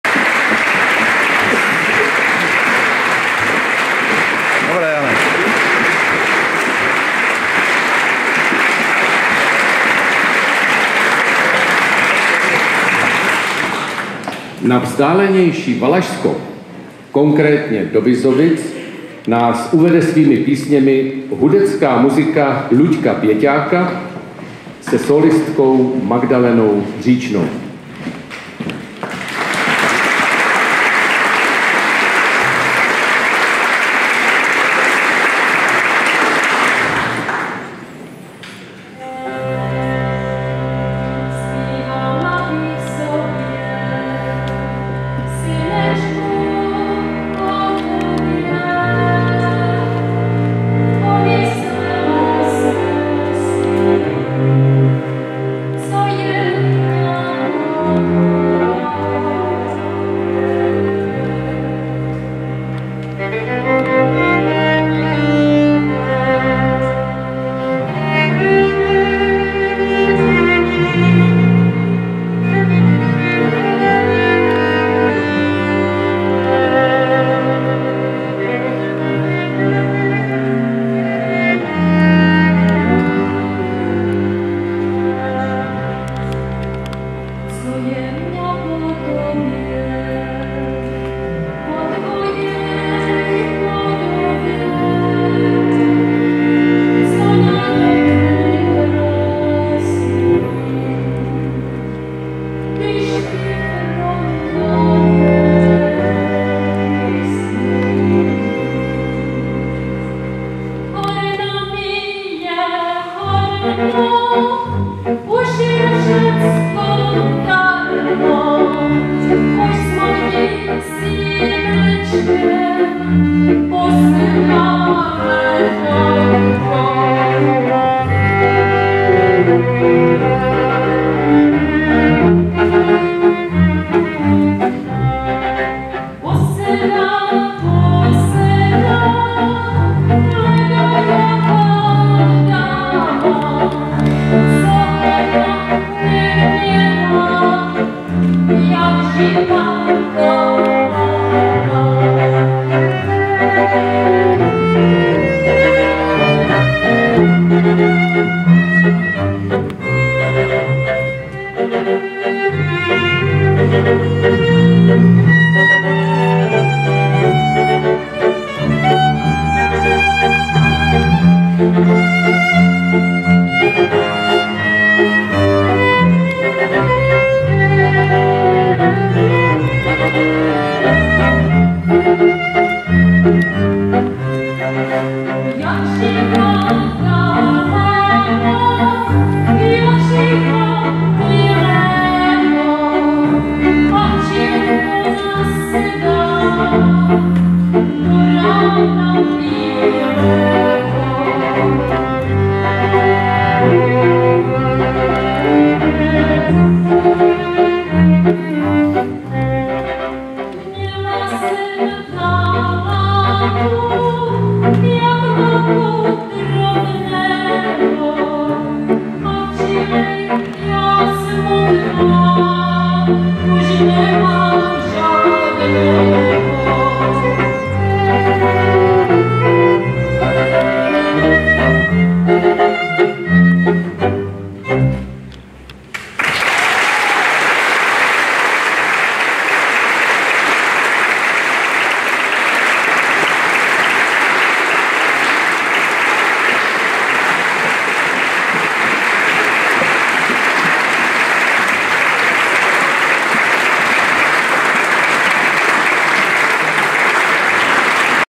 BRNO-POCTA P.FRANTIŠKU SUŠILOVI: Hudecká muzika Luďka Běťáka a Magdalena Říčná
-1. června, 19.00 hodin, Besední dům v Brně
- večer k 150. výročí úmrtí významného moravského buditele a sběratele lidových písní
-autoři pořadu: Libor Jan-Václav Štěpánek